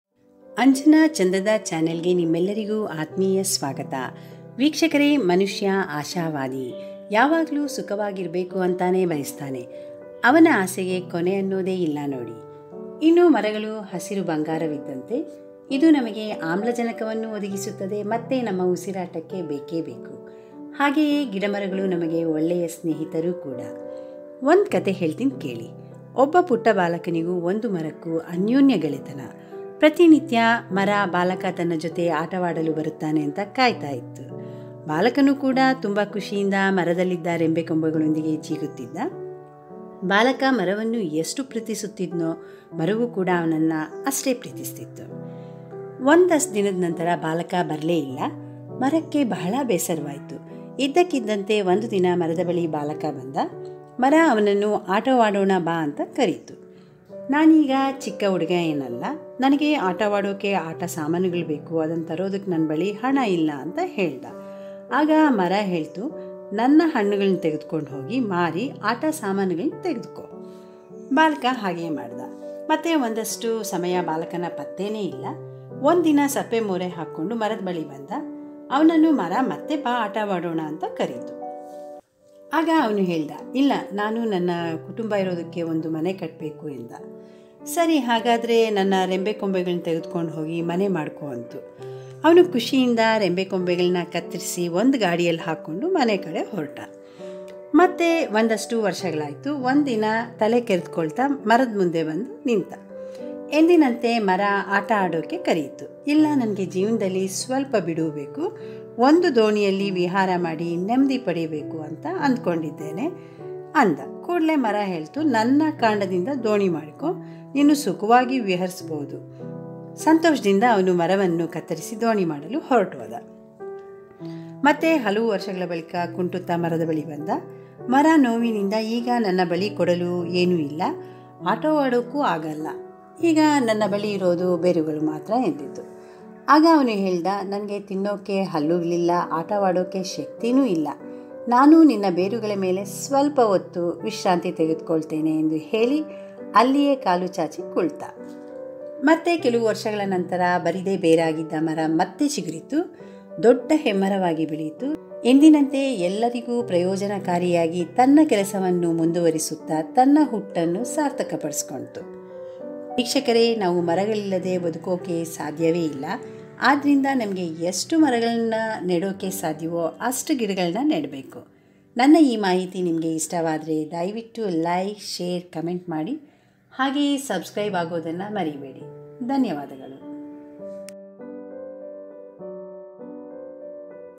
Interesting story of a tree and a boy in kannada | @anjanachandada
Hello, this is the 112th video on my channel, @anjanachandada

This video is a story covering some of the moral aspects to live a meaning full life, a story about a boy and a tree.